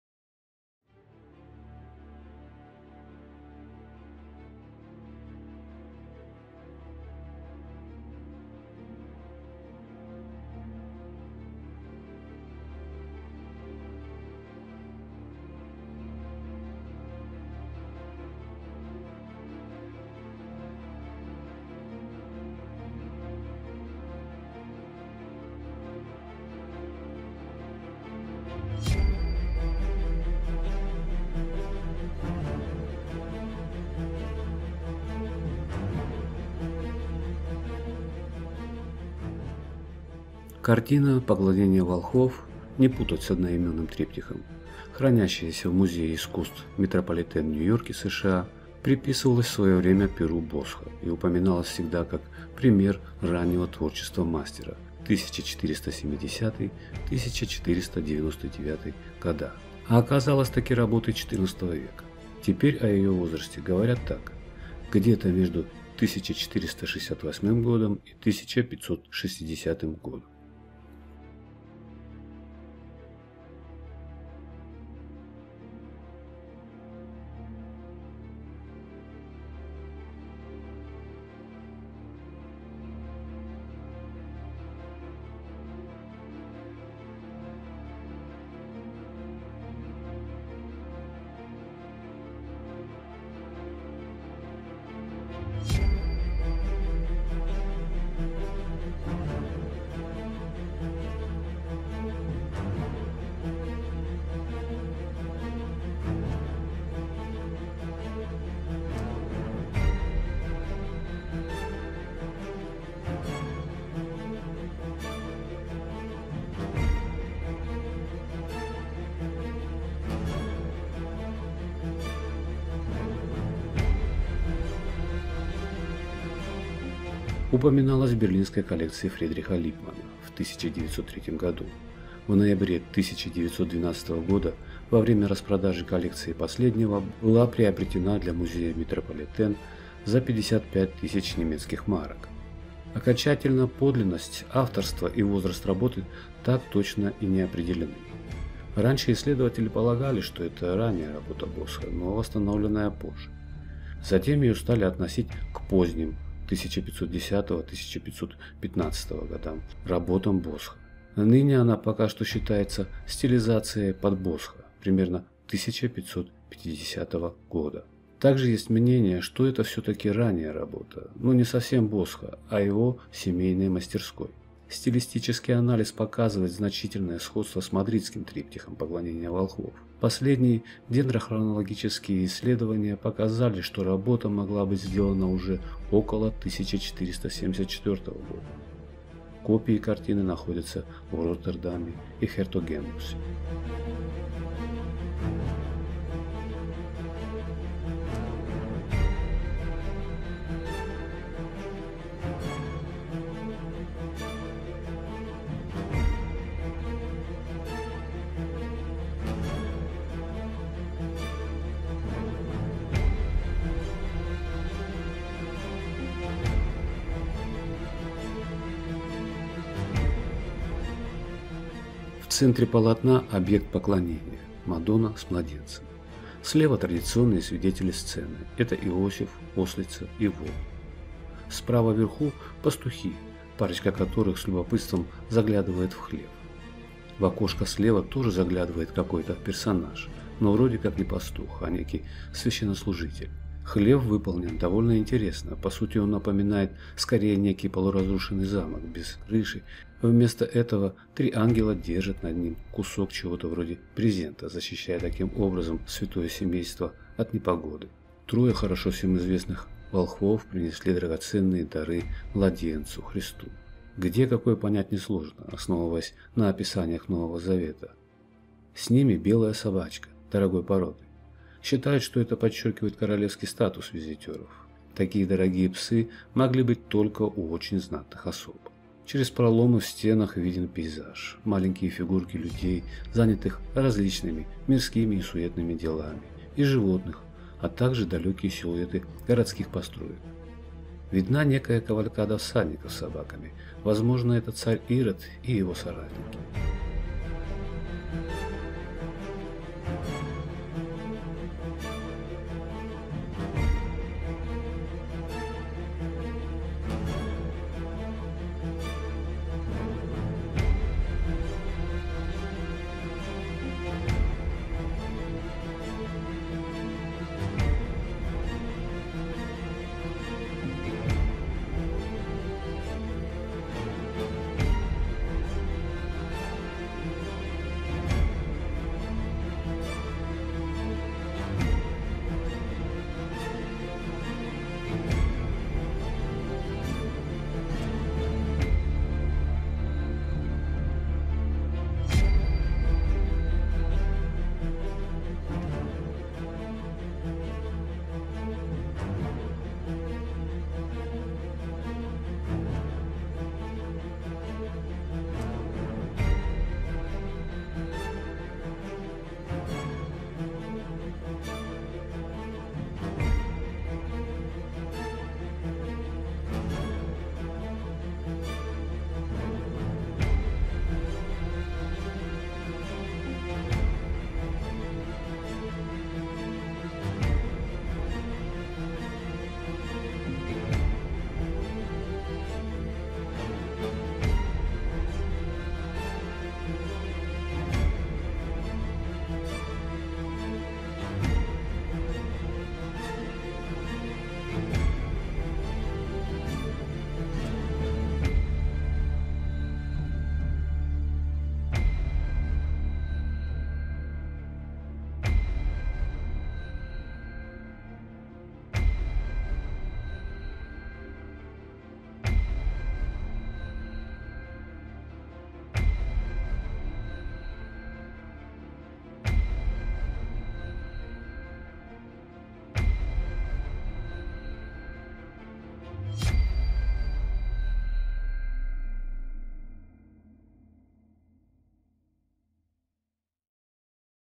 Иероним Босх. Поклонение Волхвов (The Adoration Of The Magi). Метрополитен музей, Нью Йорк
На случай блокировки youtube регистрируйтесь на моём канале на Яндекс Дзене:
В своё время произведение «Поклонение Волхвов» «The Adoration Of The Magi», находящаяся в музее искусств Метрополитен в Нью-Йорке, приписывалась перу Босха. Картина считалась примером его раннего творчества. Но оказалось, что работа эта XIV века. Её возраст около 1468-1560 гг.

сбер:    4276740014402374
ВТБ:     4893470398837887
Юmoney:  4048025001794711
Через телефон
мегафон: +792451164

 описание картины Поклонение Волхвов
Иероним Босх
Поклонение Волхвов
(The Adoration Of The Magi)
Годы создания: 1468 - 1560
Северное Возрождение
Материалы: дерево, масло
Размеры
Высота: 71.1 см (27.9 дюйма);
Ширина: 56.5 см (22 дюйма);

Местонахождение картины Поклонение Волхвов:
Метрополитен-музей, Нью-Йорк
(Metropolitan Museum Of Art, New York)